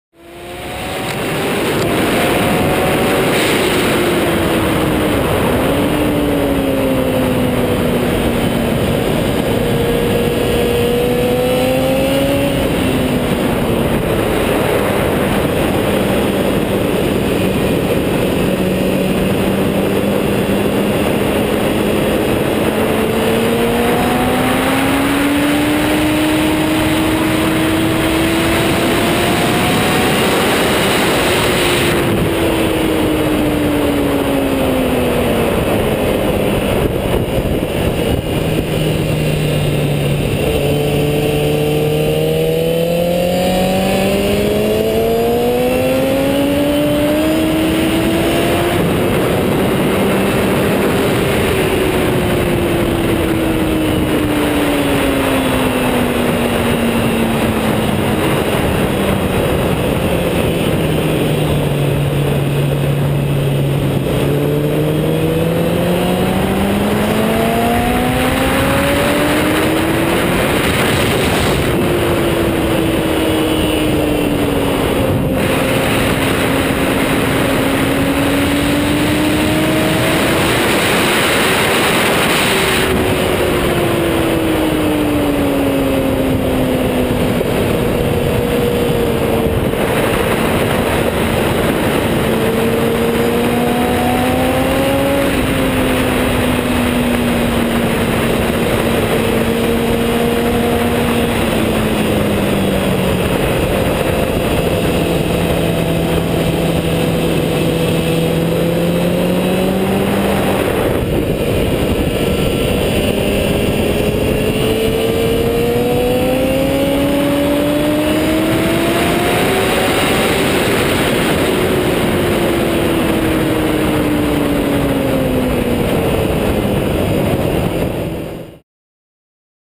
Barber072011POV.mov
a lap around Barber Motorsports Park with the new chest mount for the GoPro. Needs some work but overall it's pretty nice to see a different perspective.